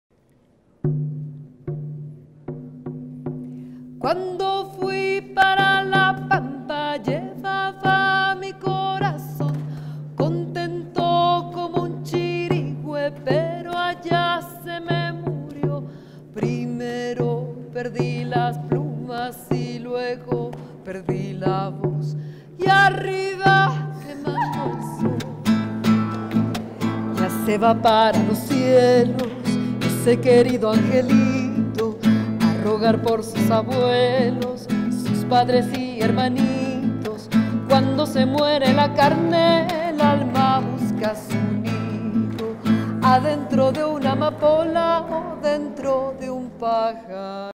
“Las Voces de Violeta”, grupo Areka Teatro.
“Las Voces de Violeta”, es una obra de teatro presentada por el grupo Areka Teatro por los 100AñosVioletaParra realizado el 23 de octubre en la Sede de la Secretaría General de UNASUR, Quito - Ecuador.